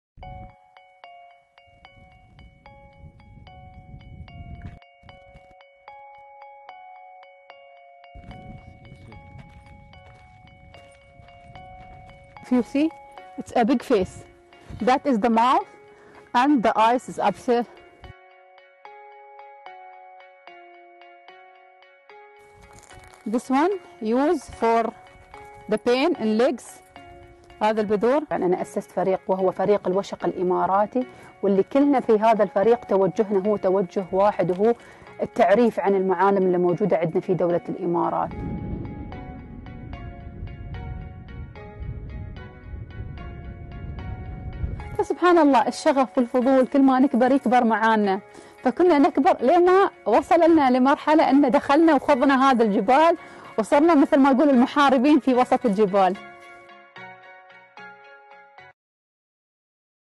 تقرير مرئي من صفحة " The national news " واختيار المغامرة الإماراتية رحاب الظنحاني كشخصية ملهمه
تقرير مرئي من صفحة " The national news "

واختيار المغامرة الإماراتية رحاب الظنحاني كشخصية ملهمه في دولة الإمارات .. والحديث عن جانب البحث والإستكشاف وأهداف فريق الوشق الإماراتي للمغامرات..

شكرا على هذا التقرير الجميل والدعم الإعلامي في مسيرتنا..

شكرآ للاستاذه بلقيس ومصورنا المبدع سهيل..

ملاحظة :يمنع النقل والتداول دون حفظ حقوق الناشر ..